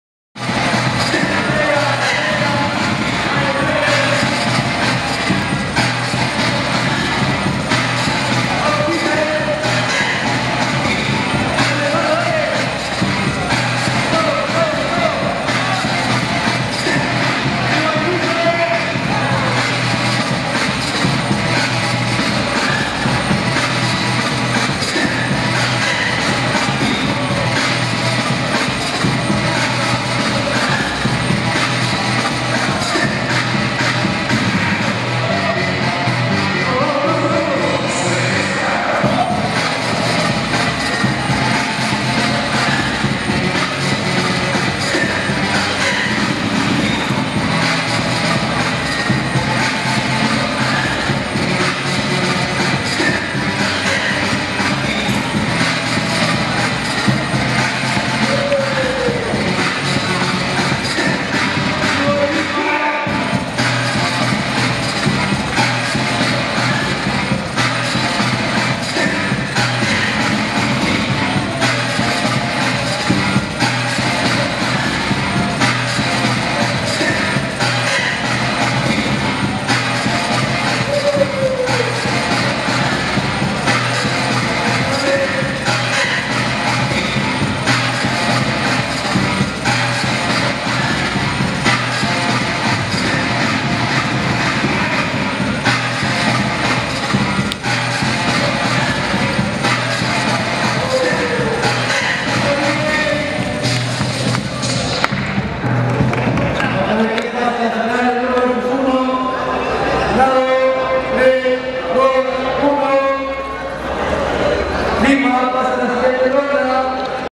C-fer VS Lima 11 º ANIVERSARIO DE LATIN BREAK CREW
11 º ANIVERSARIO DE LATIN BREAK CREW

Evento organizado para celebrar los 11 años de Union de una gran Familia gracias al Hip Hop es por este motivo que la familia brinda una gran fiesta con la finalidad de confraternizar mas a las Crews de nuestro pais y saber mas de nuestra cultura Hip-Hop, habran batallas de Breaking,Cyphers, Jams

! DJ !

DIEGO BREAK EN LAS TORNAS!

MODALIDAD :

* Crew Vs Crew ( 8 Vs 8 )

* Bboys ( 1 VS 1 )

* Power Move ( 1 vs 1 )

JURADOS :

* Bboy CHUMBI ( Latin Break )

* Bboy FakinJam ( Original Bboys)

* Bboy Lupi ( Combinacion Perfecta )

*Erick Soul One ( Life Style Crew)

*Yordi Style ( Team Norte Malcriados)